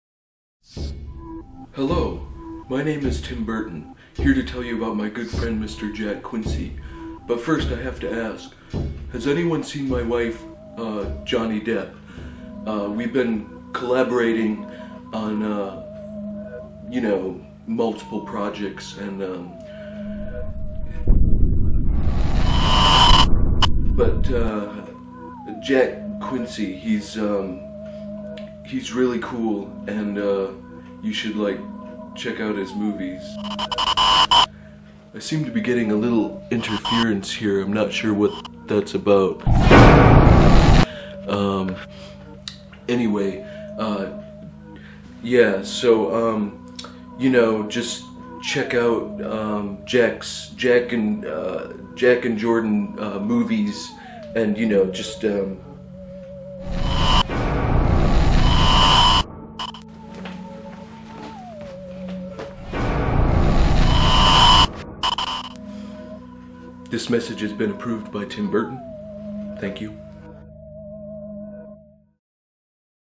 A Message From Tim Burton
My good friend Mr. Burton dropped by to say a quick message and promote my cool movies. Happy Halloween!